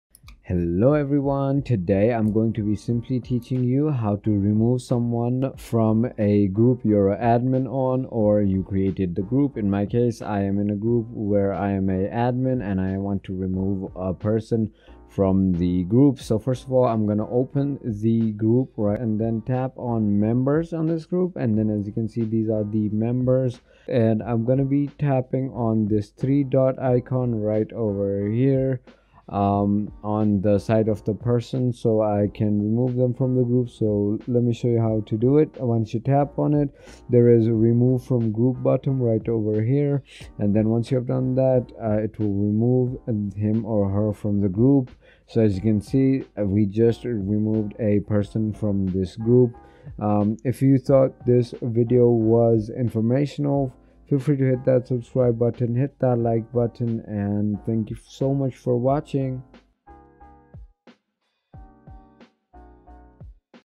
How To Remove Member From Messenger Group
In this turorial video I will quickly guide you through step by step process on how to remove add members from Facebook messenger group.